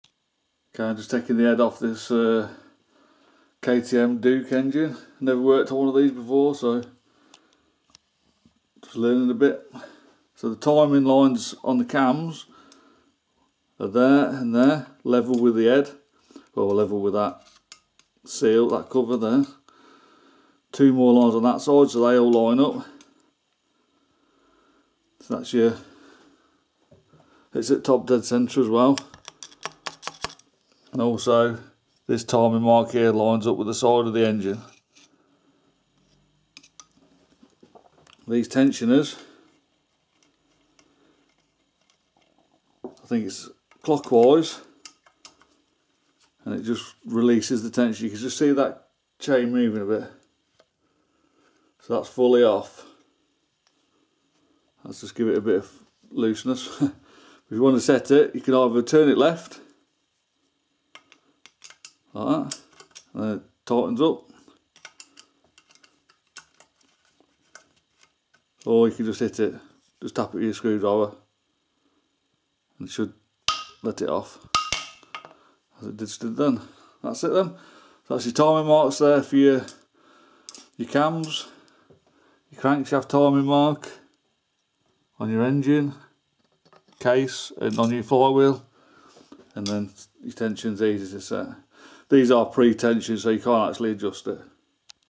KTM Duke RC 125 engine timing marks , setting top dead centre TDC and chain tensioner
Just have a look inside a KTM Duke RC 125 engine , looking at interesting parts and learning. Im not experienced on these just looking and learning myself so use your manual for specs tests anf torques etc.